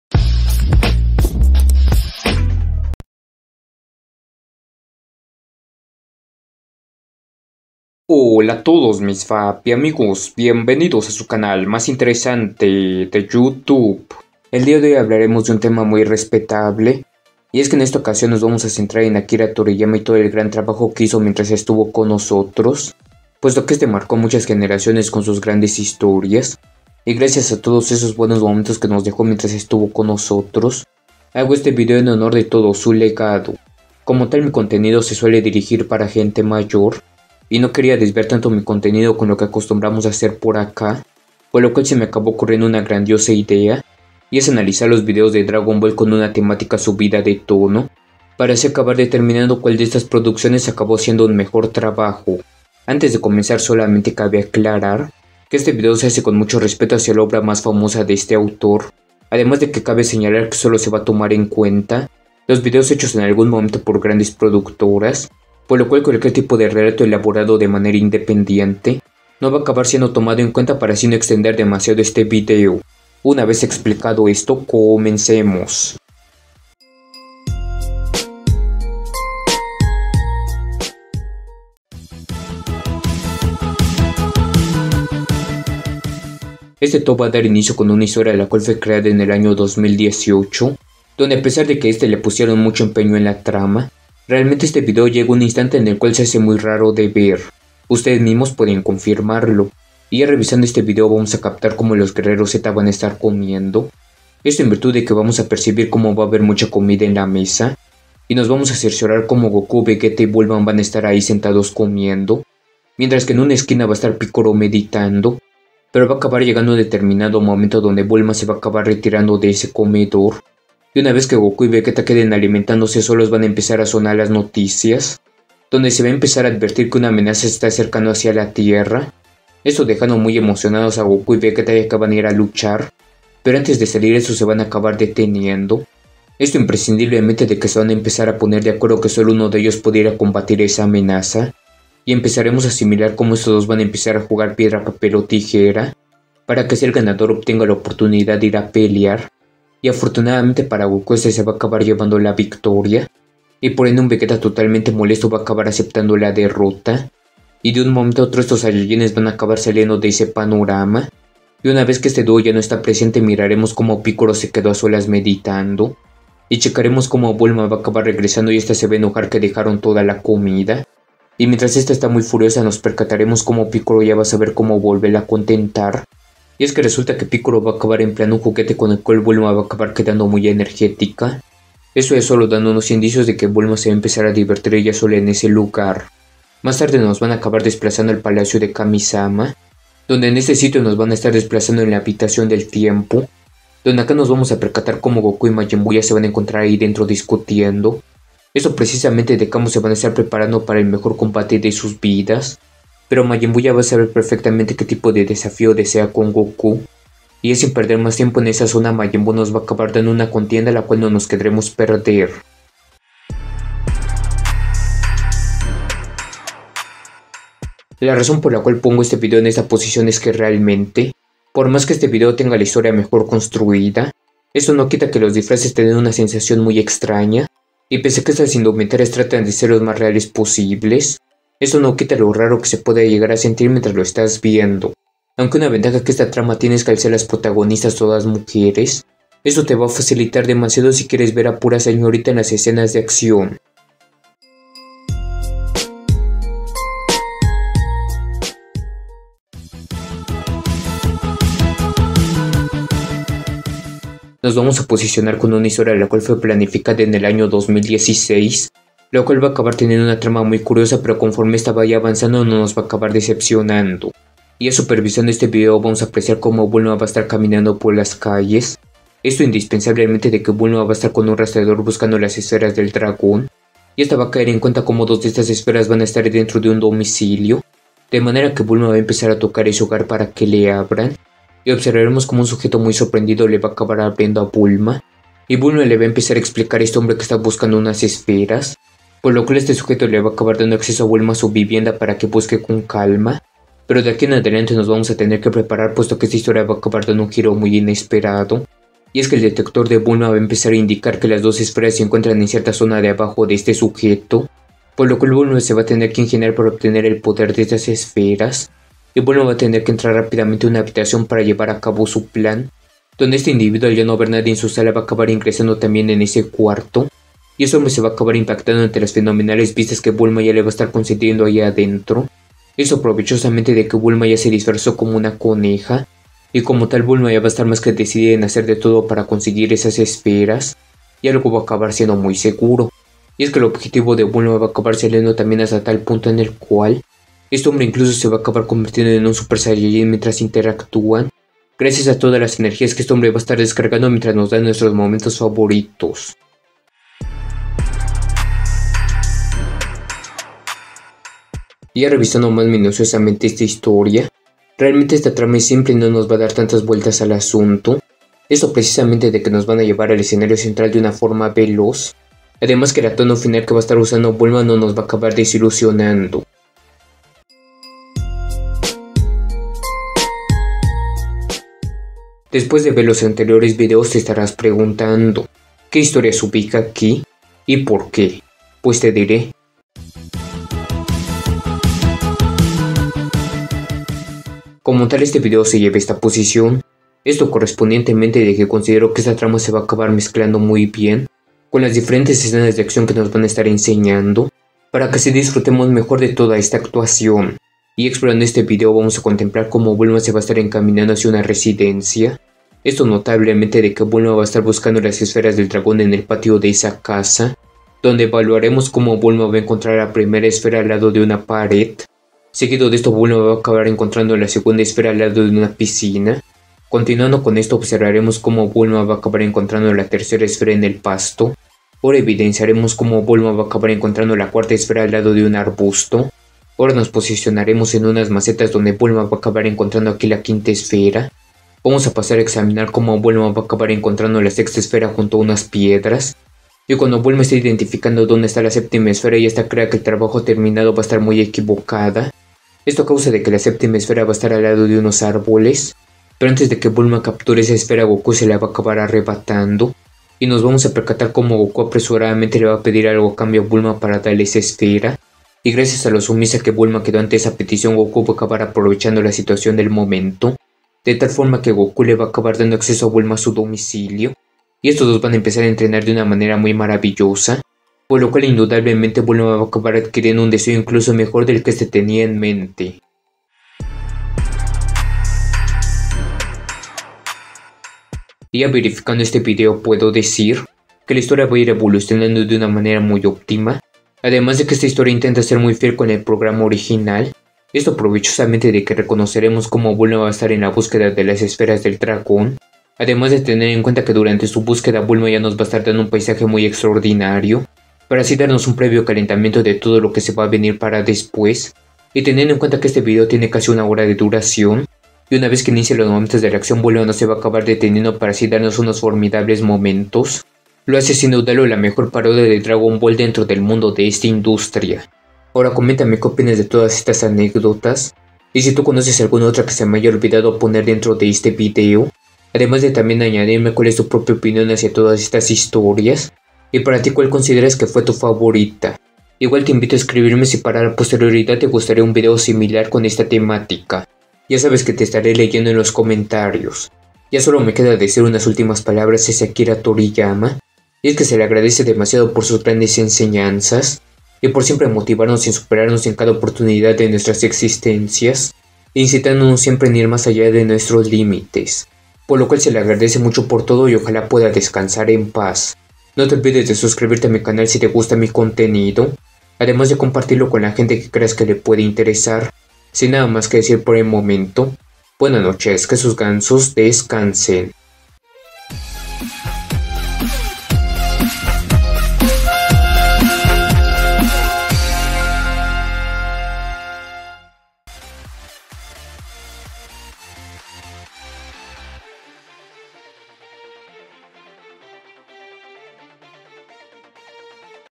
Analizando las parodias n0p0r de Dragon Ball
Hola a todos, el día de hoy les traigo un nuevo video, en esta ocasión en honor a Akira Toriyana hablaremos de las parodias hechas por las grandes productoras hacia su serie más famosa Dragon Ball, no te olvides de suscribirte a mi canal si te gusta mi contenido, además de compartirlo con la gente que este aburrida, les deseo a todos un excelente día.